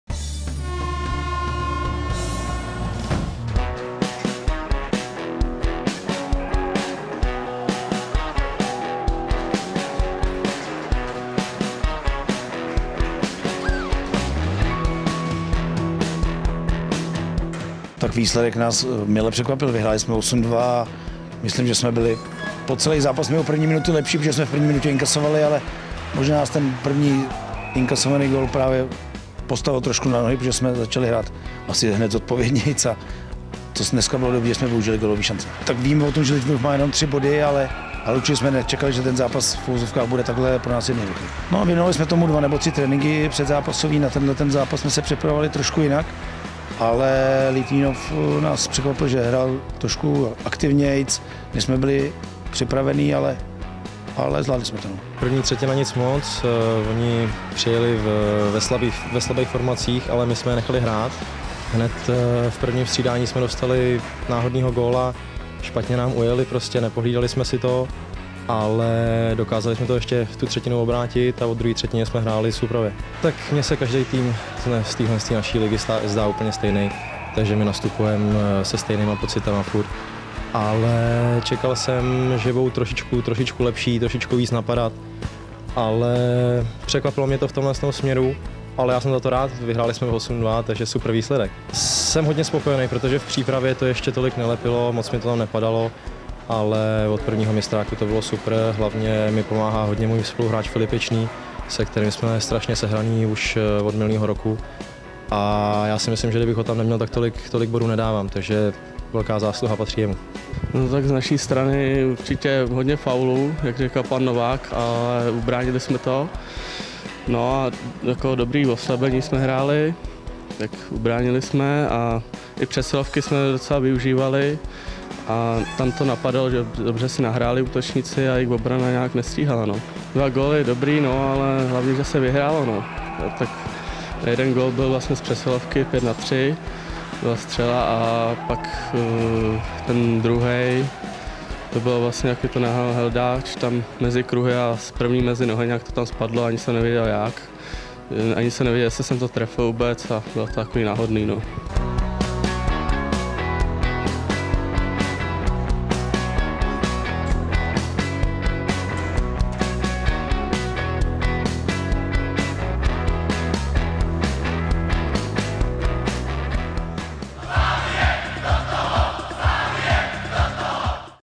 Rozhovor s juniory po výhře nad Litvínovem
Sešívaní junioři si ve čtvrtek s chutí zastříleli, osmi góly zválcovali Litvínov​ a v tabulce DHL extraligy se vyhoupli na první místo. Podívejte se na pozápasové ohlasy s hlavním trenérem Petrem Novákem, dvougólovým obráncem Karlem Drahorádem a čtyřbodovým útočníkem Viktorem Nestrašilem.